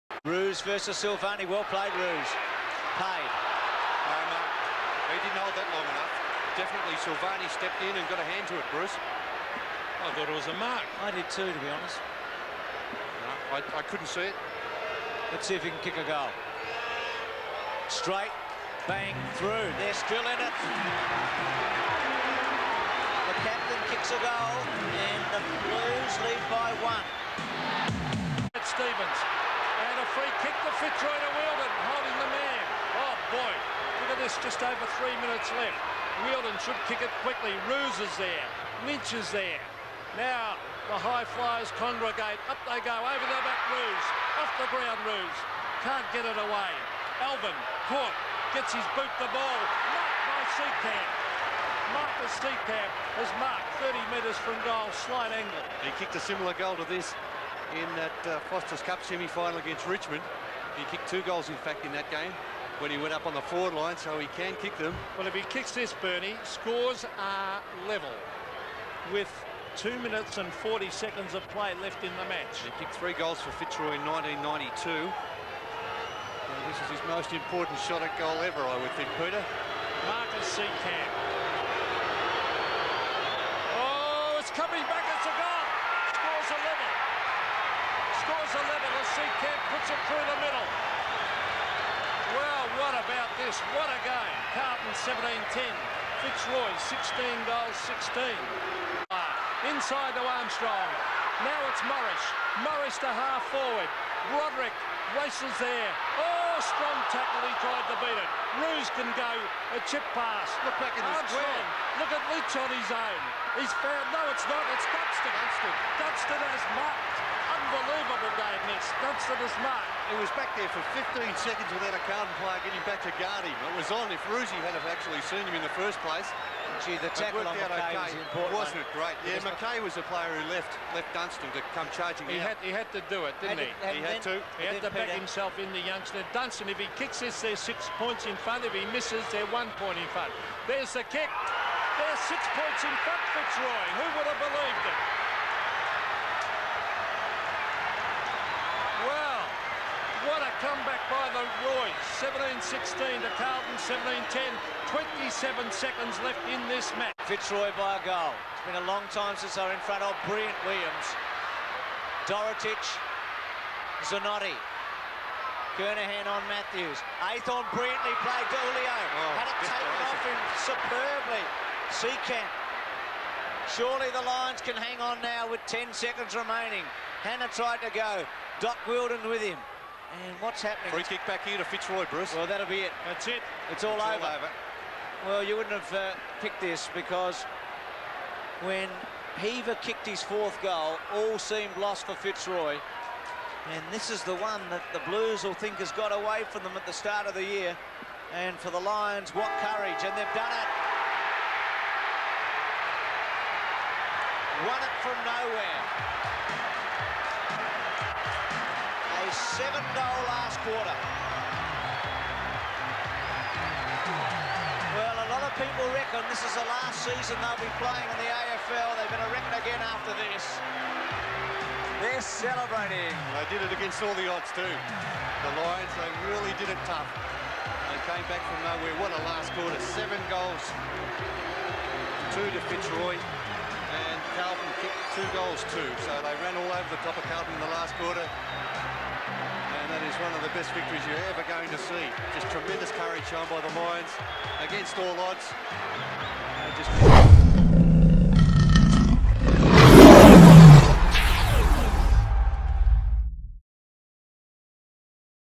TBT: Fitzroy vs Carlton
This weeks Throwback Thursday is Carlton vs Fitzroy in 1993.